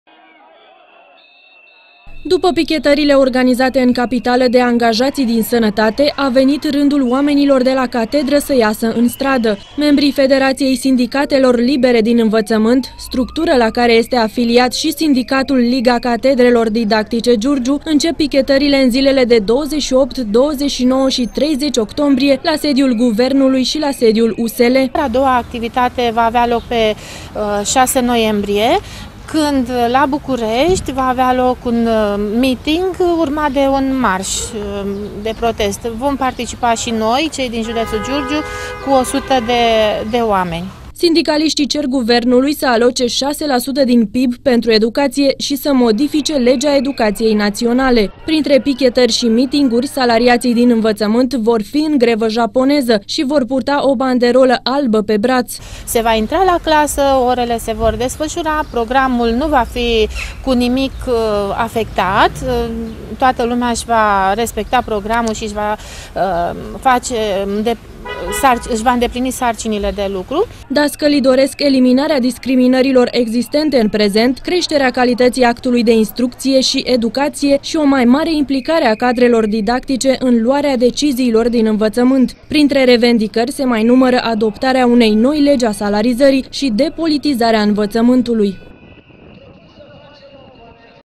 DASCALII, IN GREVA JAPONEZA
De la sfârșitul acestei luni, profesorii ies în stradă și cer să le fie respectate drepturile. Cadrele didactice din toată țara, la care li se alătură și profesorii giurgiuveni, vor picheta sediul Guvernului, urmând ca în luna noiembrie să organizeze în capitală un miting de amploare și un marș. În aceeași perioadă, în toate unitățile de învățământ, dascălii vor fi în grevă japoneză, purtând o banderolă albă pe braț.